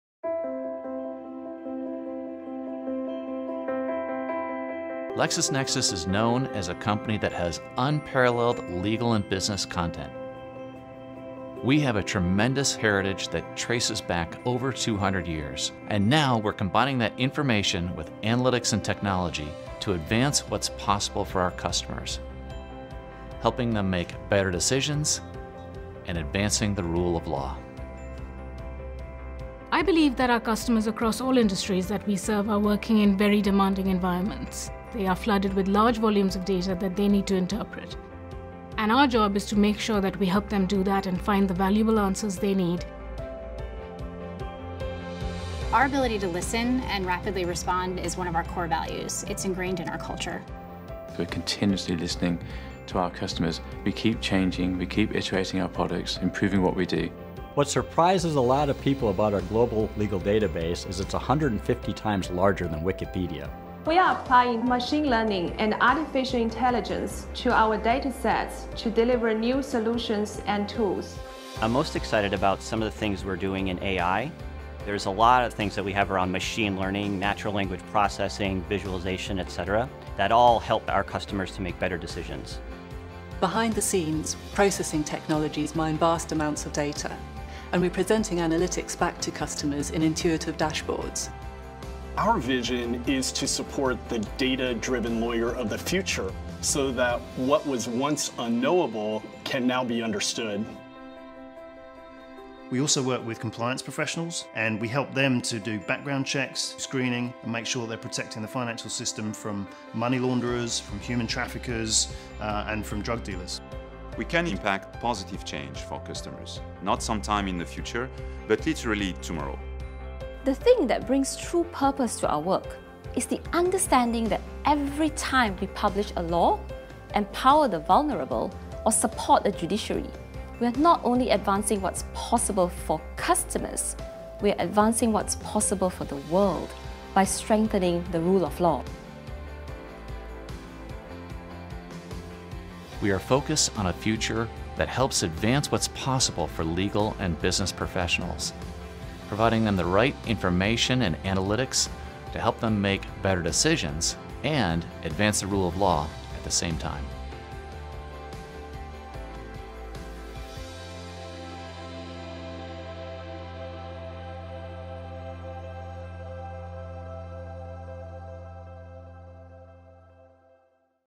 LexisNexis Legal & Professional - Advancing What's Possible
LexisNexis Legal & Professional is combining unparalleled legal and business information with analytics and technology to advance what’s possible for the way our customers work and to advance what’s possible in the world by strengthening the rule of law.